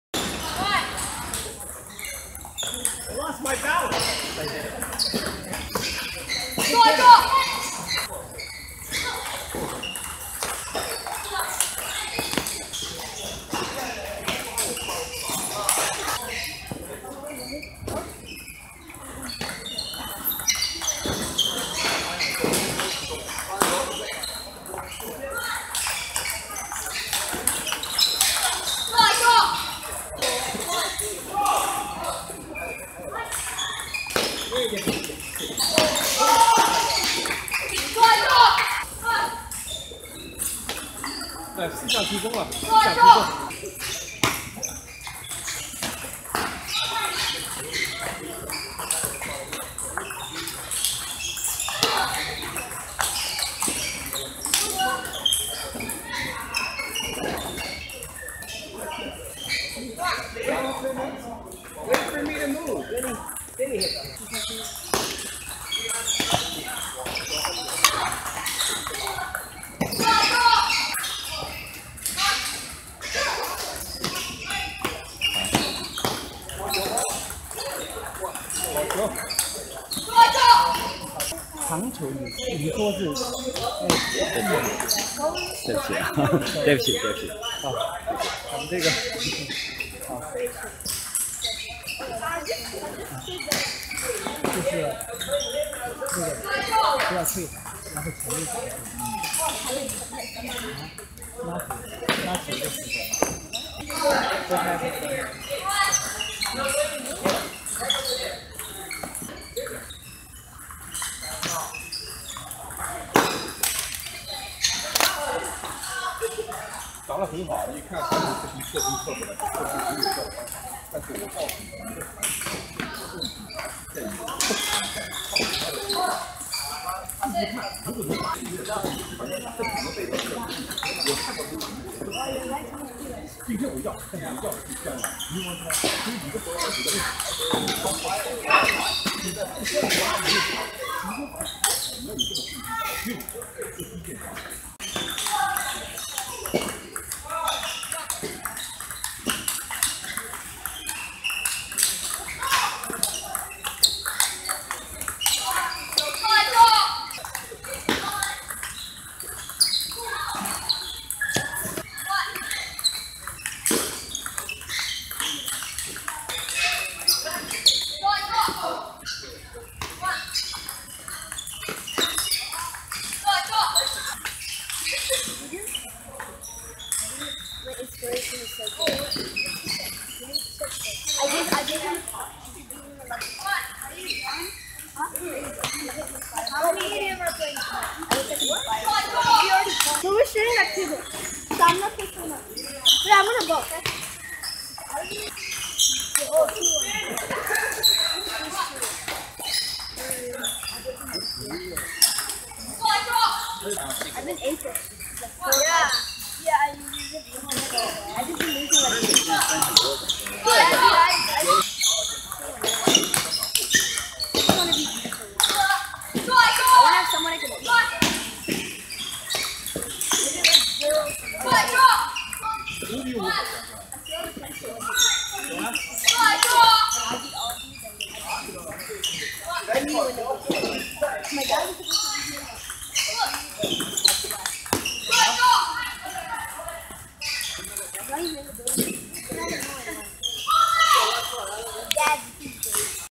Little Jonathan (1608) up against towering Rishi (1650)
Jonathan: #41 USATT Mini-Cadet Boys (12 and under)
      #125 USATT Cadet Boys (14 and under)
Rishi -  6'4" #368 USATT Junior Boys (18 and under)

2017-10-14 at ICC table tennis league